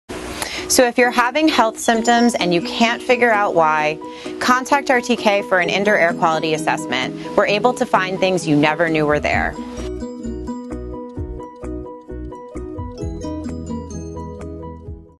Should I Test My Indoor Air Quality?
If you’ve been feeling sick without explanation or without a known cause, have your indoor air tested for VOCs (volatile organic compounds). Here’s why:

VOCs are toxic vapors that are off-gassed from man-made materials, and everyday household (and workplace) items. VOCs cause poor indoor air quality, commonly referred to as “indoor air pollution.” Did you know that the air indoors can be up to 100x more polluted than the air outside? VOCs can be very dangerous to your health and can be toxic. Err on the side of caution and have your IAQ tested.